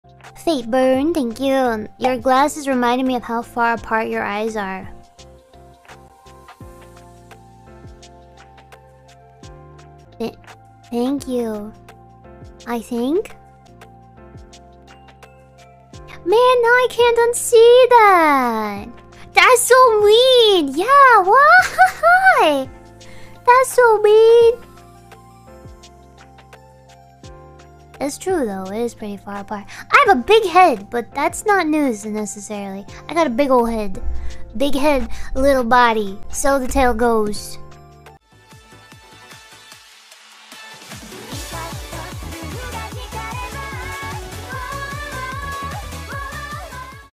𝐆𝐮𝐫𝐚 𝐠𝐨𝐭 𝐭𝐨𝐥𝐝 𝐡𝐞𝐫 𝐞𝐲𝐞𝐬 𝐚𝐫𝐞 𝐬𝐨 𝐟𝐚𝐫 𝐚𝐩𝐚𝐫𝐭 |Hololive EN
ED ART by @Tkminnm from Twitter -

BGM used
DOVA-SYNDROME